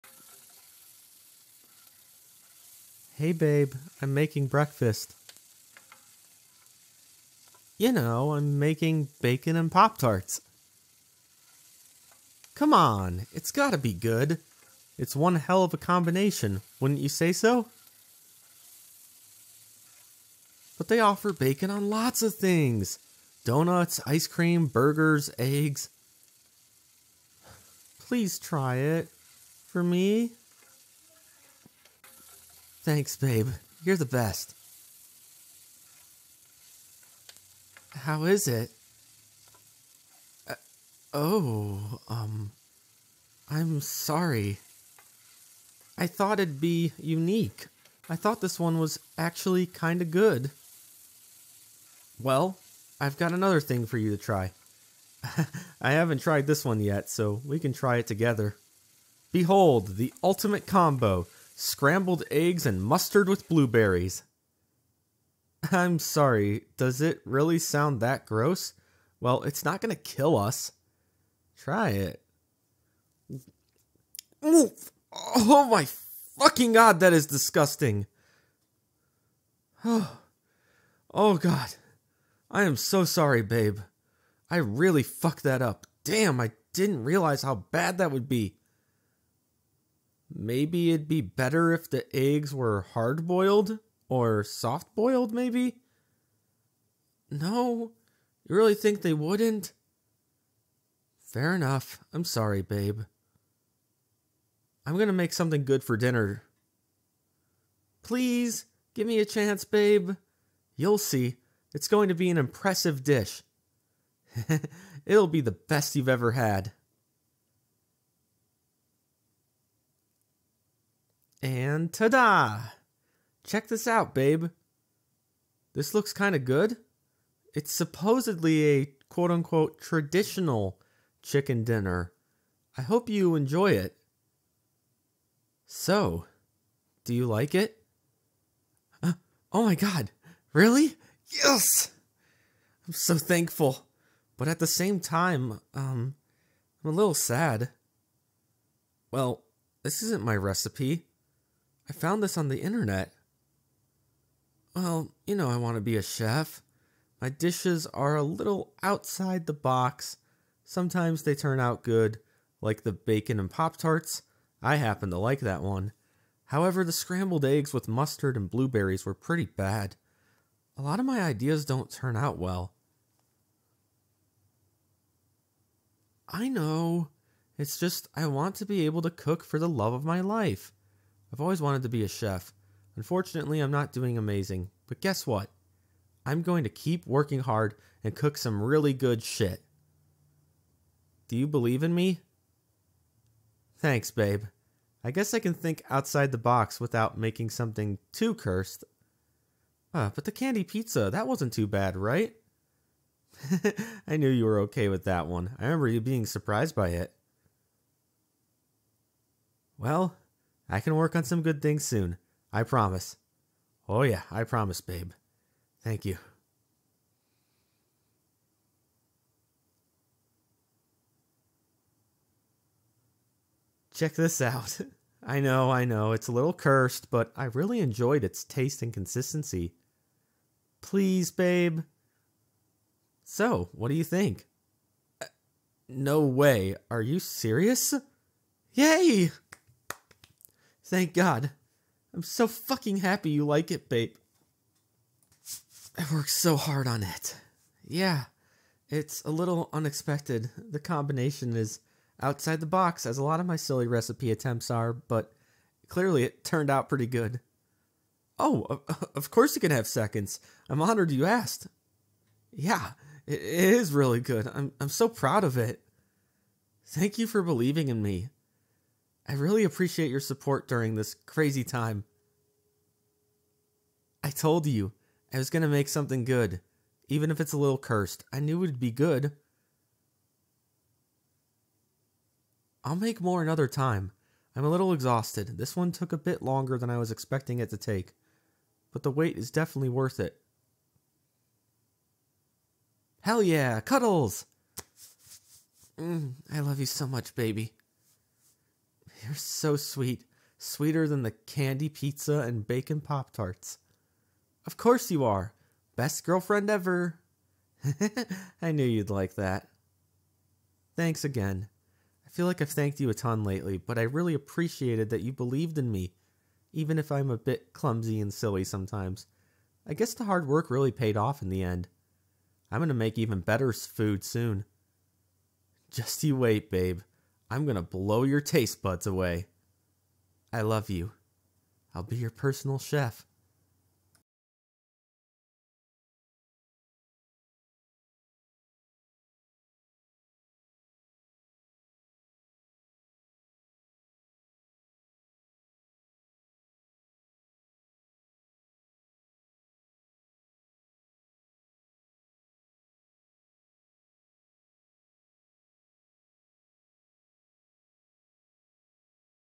Clumsy Chef Boyfriend Makes Cursed Food [M4F] [Clumsy] [Silly] [Boyfriend]
Character made using VRoid Studio (Hair asset by CarolunaVtuber on Twitter)
ALL CHARACTERS ARE 18+ AND FICTIONAL!